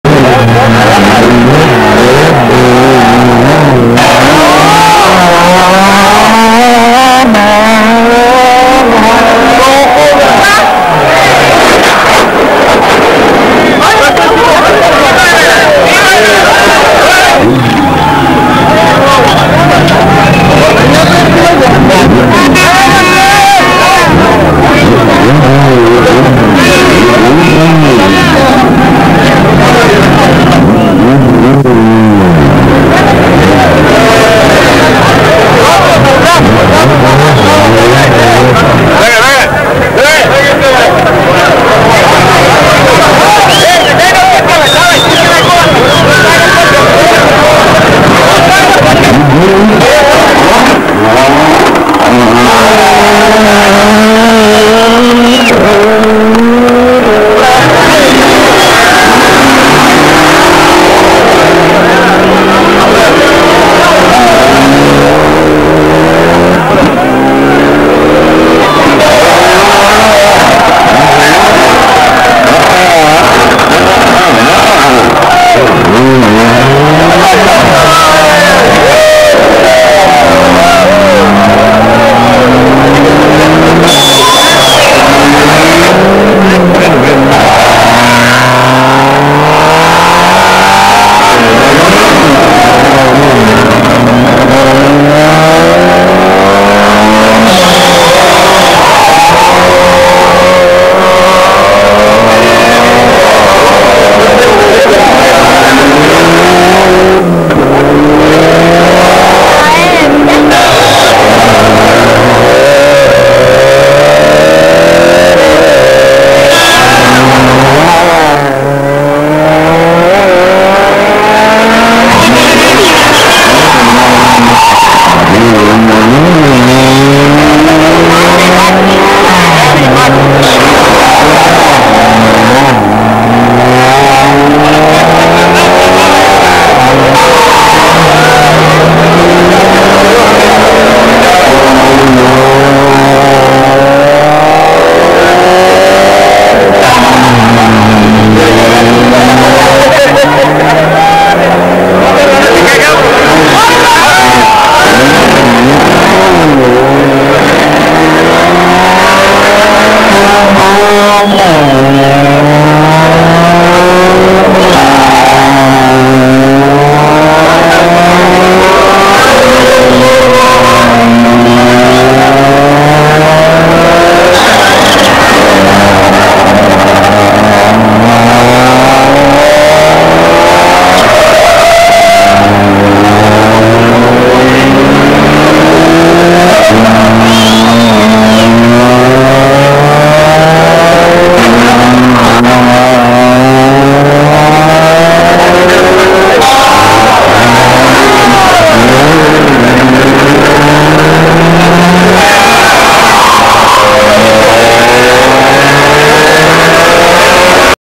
Rally Albena 1999 - SS14 Stara reka 2
Unique footage from SS Stara reka and Dimitar Iliev crash, Rally Albena 1999, European Rally Championship Coeff. 20